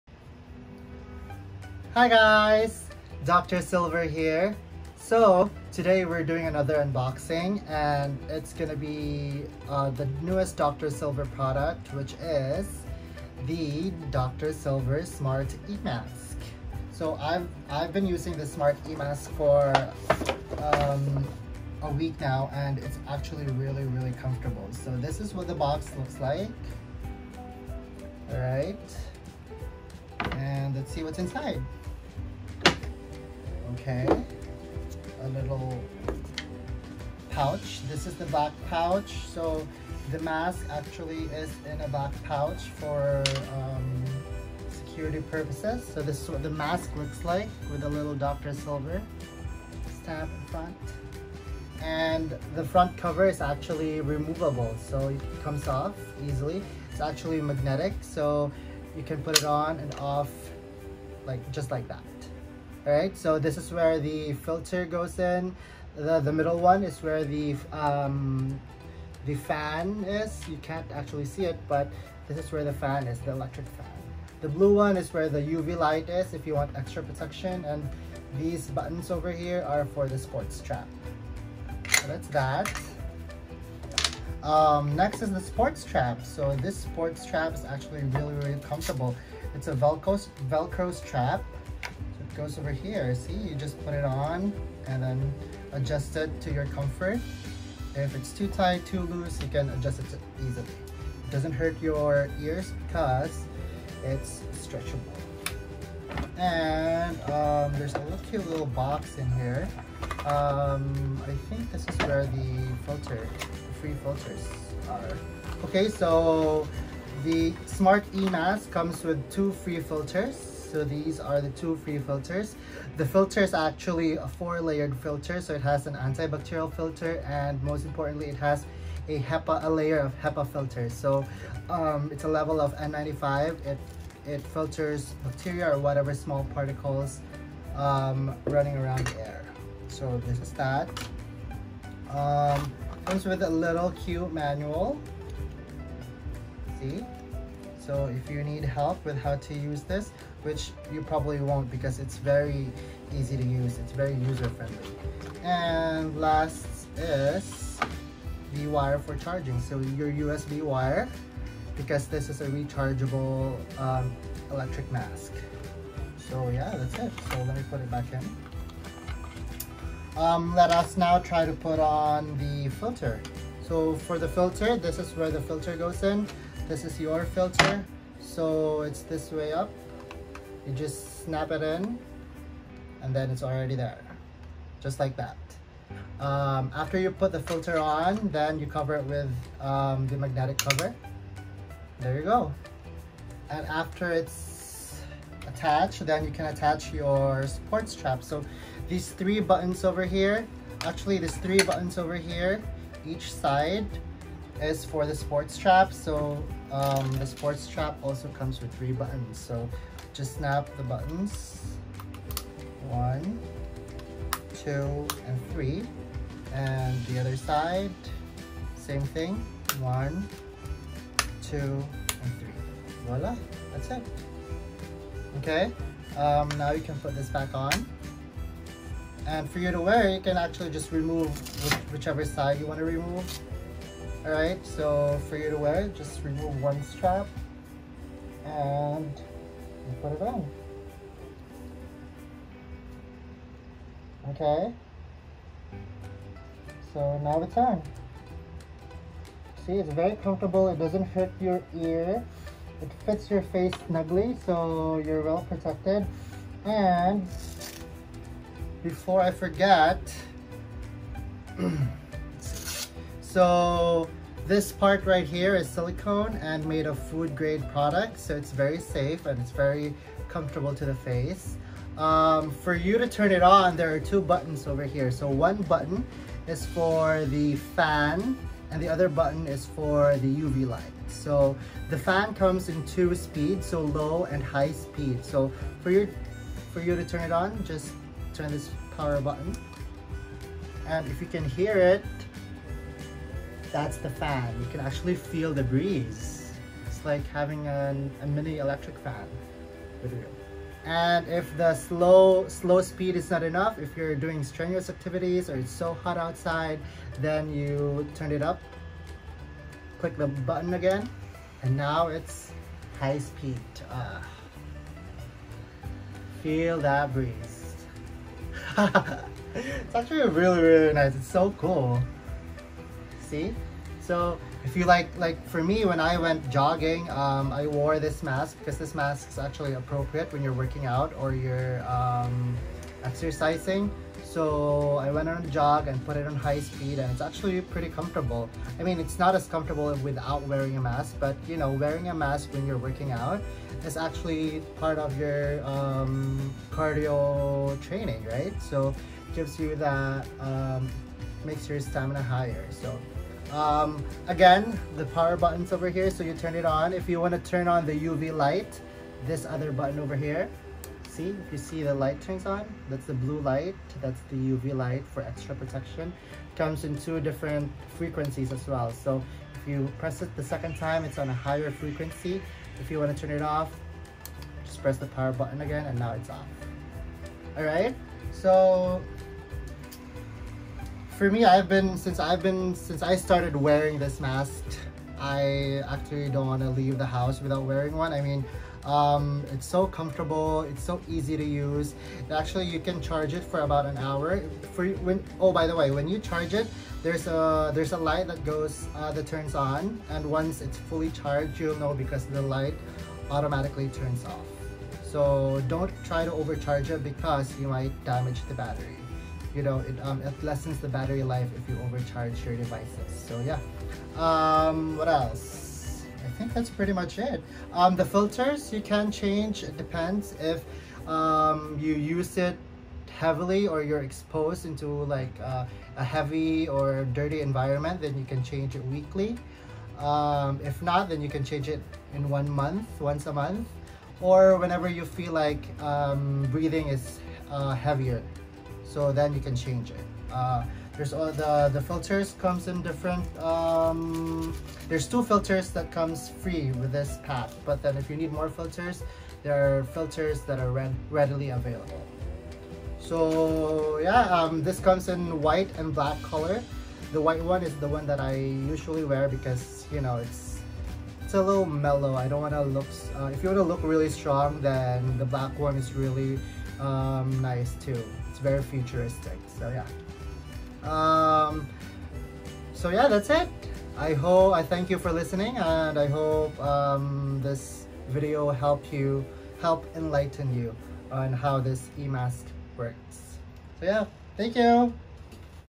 Half-face Powered Air Purifying Respirator (PAPR) / Dr. Silver Smart Electric Mask
SMART  E-MASK unboxing 🎁

☑️ Half-face powered air purifying respirator (PAPR)
☑️ Built-in motor fan for easy breathing
☑️ Four layer filter protection with HEPA filter
☑️ Adjustable and comfortable sport strap
☑️ USB rechargeable
☑️ Sleek design and very comfortable to wear
☑️ Seal is made of silicone from food-grade materials
☑️ Environmentally friendly plastic materials used throughout the body
☑️ Can be used for sports and exercise
☑️ Colors: Black and White
☑️ Comes with 2 FREE filters